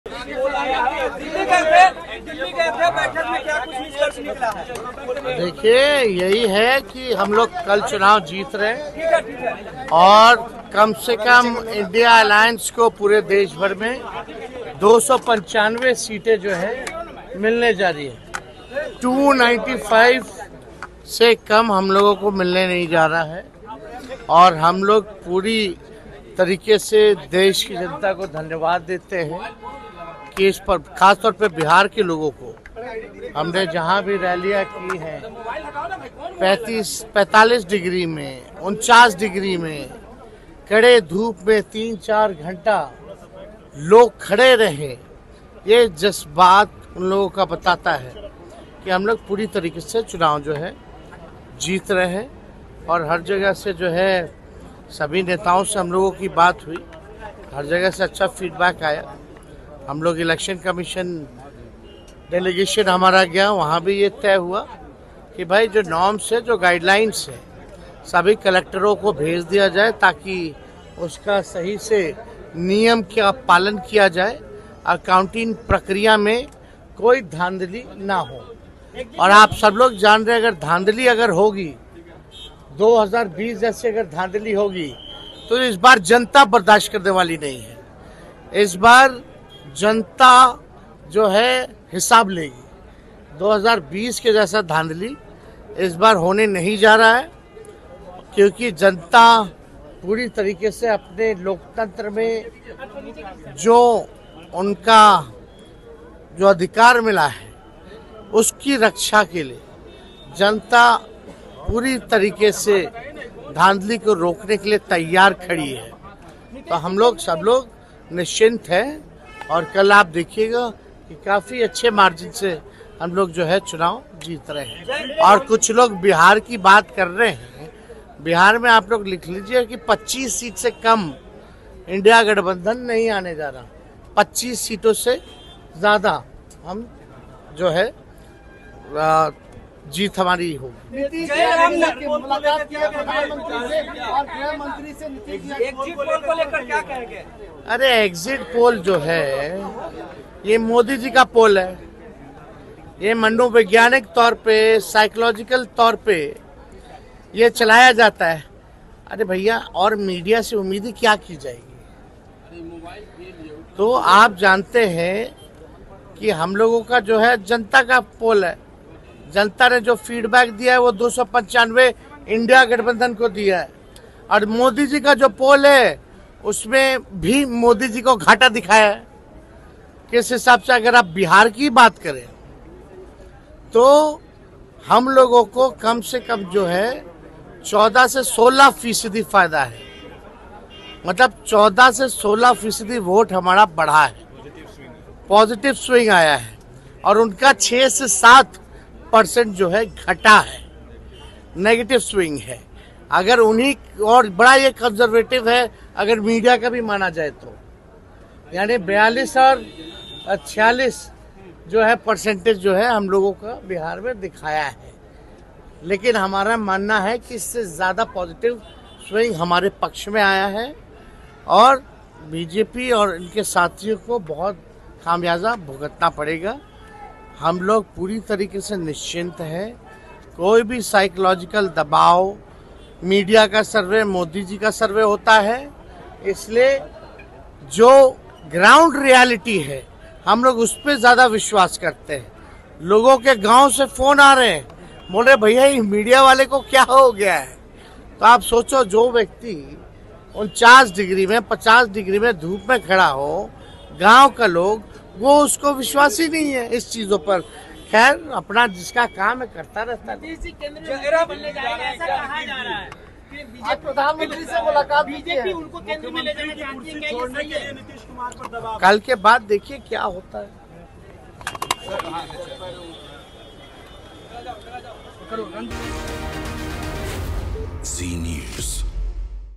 Tejashwi Yadav on Exit Poll: एग्जिट पोल पर तेजस्वी यादव का बड़ा बयान | Lok Sabha Results 2024 | Bihar
On the exit poll, Tejashwi Yadav said that this is Modi ji's poll. It is run psychologically. What to expect from the media. This is our public's poll. The feedback given by the public is 295+ Modi ji's moment. Modi ji has been shown inferior. If we talk about Bihar, we have at least 14 to 16 percent advantage. 14 to 16 percent votes are bigger for us, they have 7% deficit. We believe that the positive polls are in our favor. We are absolutely sure that no psychological pressure can shake the public.

एग्जिट पोल पर तेजस्वी यादव ने कहा कि यह मोदी जी का पोल है मनोवैज्ञानिक तौर पर यह चलाया जाता है मीडिया से उम्मीद ही क्या किया जाए हम लोगों का जनता का पोल है जनता ने जो फीडबैक दिया है 295+ मोदी जी का जो पल है उसमें मोदी जी को घटा दिखाया है बिहार की बात किया जाए तो हम लोगों को कम से कम 14 से 16 फ़ीसदी फायदा है 14 से 16 फ़ीसदी वोट हम लोगों का बड़ा है उनका 7% घाट है हमारा मानना है कि हमारे पक्ष में पॉजिटिव पोल में आ रहा है हम लोग पूरी तरीके से निश्चित है कोई भी साइकोलॉजिकल दबाव जनता को नहीं हिला सकती। मीडिया का सर्वे मोदी जी का सर्वे होता है इसलिए जो ग्राउंड रियलिटी है हम लोग उसे पर विश्वास करते हैं लोगों का फोन आ रहा है गांव से मीडिया वाले को क्या हो गया है जो व्यक्ति 49 और 50 डिग्री में धूप में खड़ा हो उसको विश्वासी नहीं है इस चीज पर। सत्य की जीत तय है जनता की जीत तय है।

Tejashwi Yadav on Exit Poll: नेता प्रतिपक्ष तेजस्वी यादव का बयान | Lok Sabha Election 2024 Results | Bihar Politics